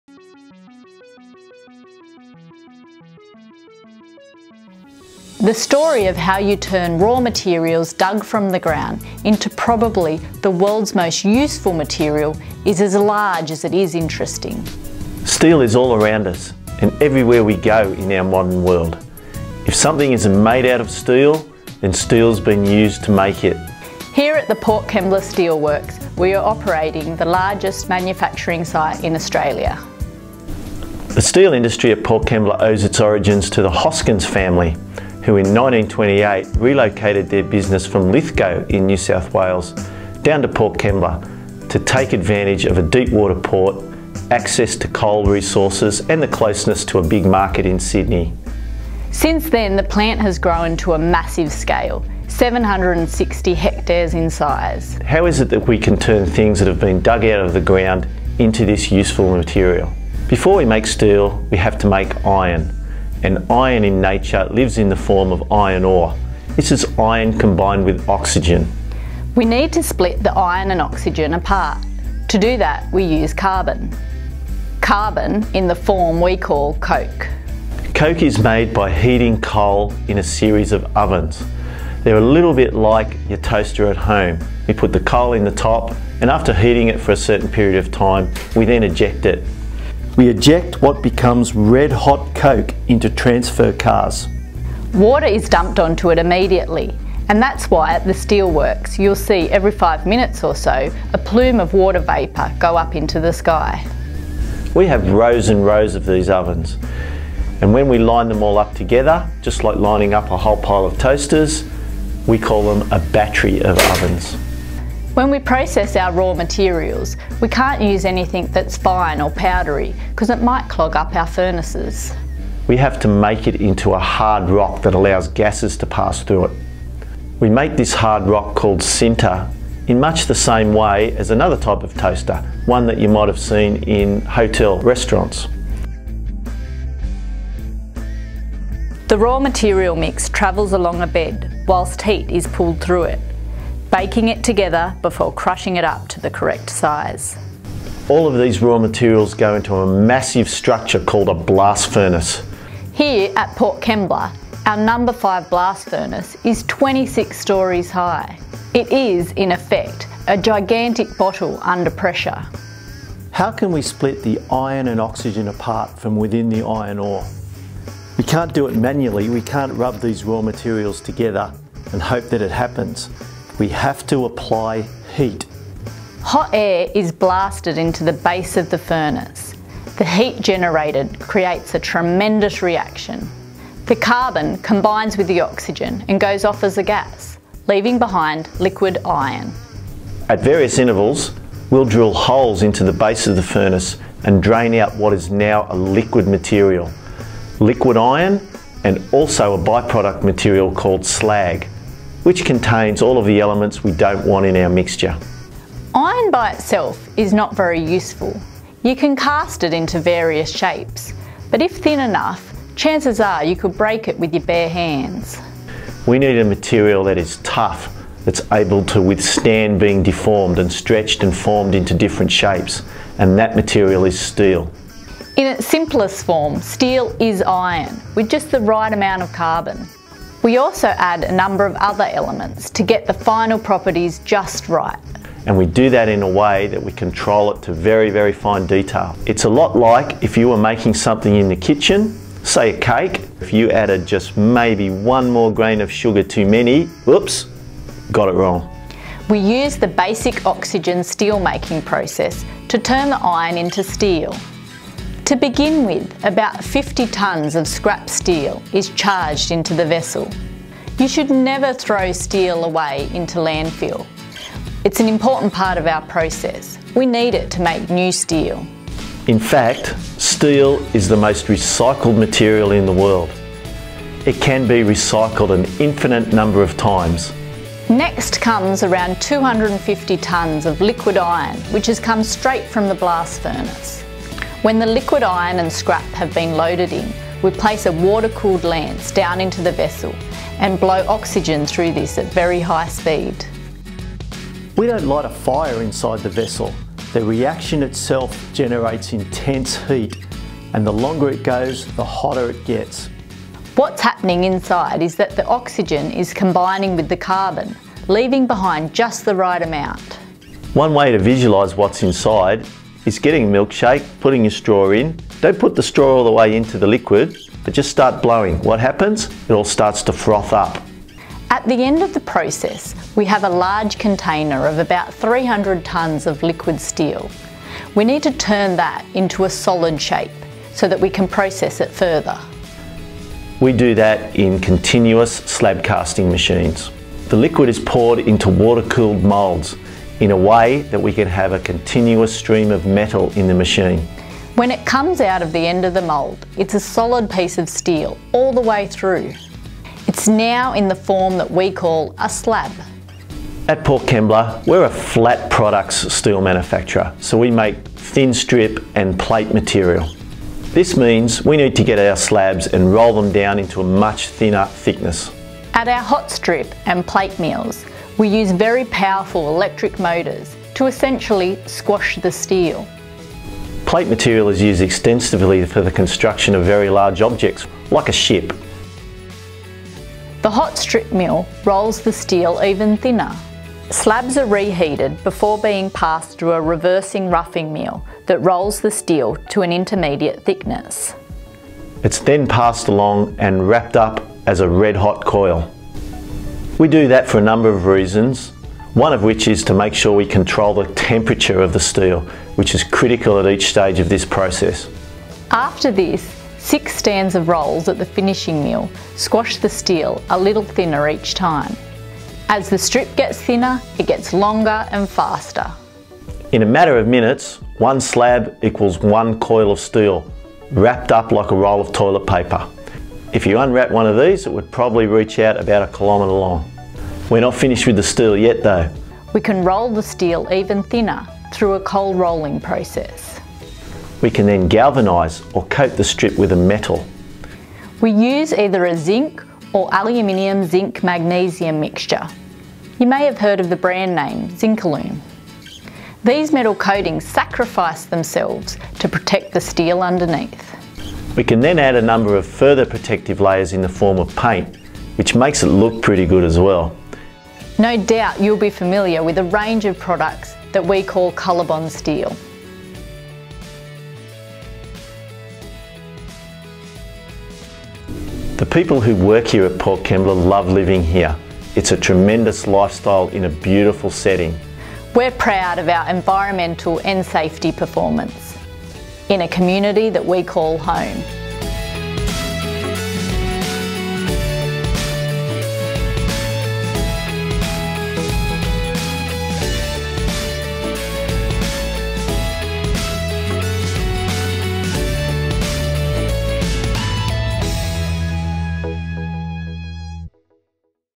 The Story of Steel — Port Kembla Steelworks (2018)
Happy 90th birthday, Port Kembla Steelworks! No. 1 Blast Furnace was first lit on 29 August 1928, signalling the start of iron- and steel-making at Port Kembla. In this video, you'll hear an explanation of the steelmaking process.

To see a newer version of The Story of Steel with updated imagery and our refreshed company brand, visit the following